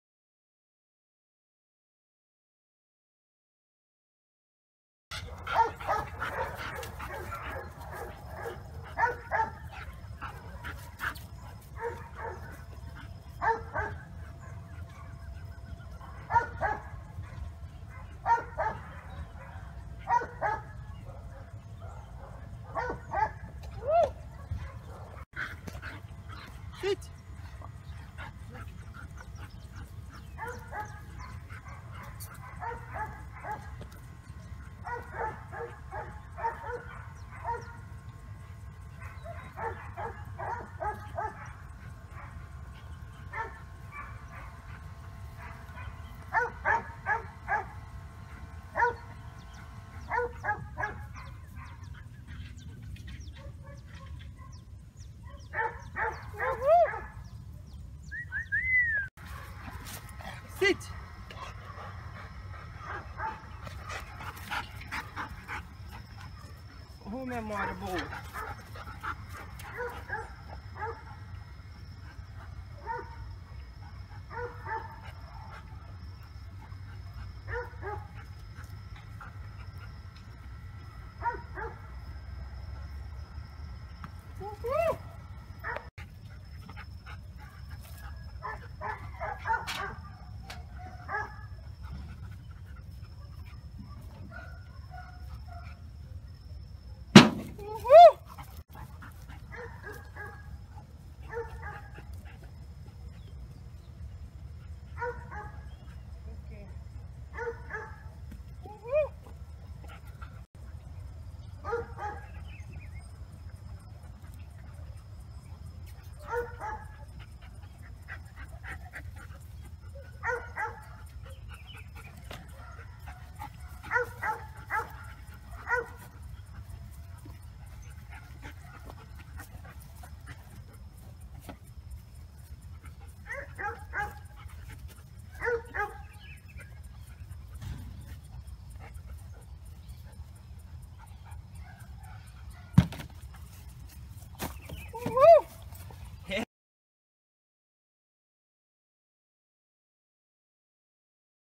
K9 INDIA BR CARAÍBAS
K9 INDIA BR CARAÍBAS PASTOR ALEMÃO 12 MESES TREINADA PARA DETECÇÃO À VENDA NO CANIL CARAÍBAS